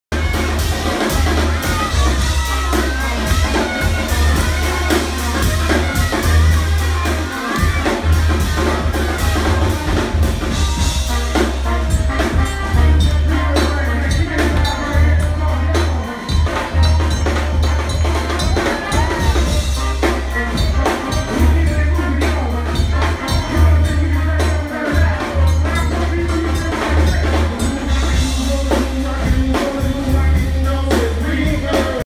High five umbrella dancers at Rebirth Brass Band (Durham, NC)
With an estimated audience of 2,750, Durham Central Park hosted more patrons than a sold out DPAC show--all for the love of summer, CreatingCommunity and Grammy Award-winning REBIRTH BRASS BAND. Talent buying, lighting and sound production services via Durham-based Sonic Pie Productions. We LOVE the high five umbrella dancers.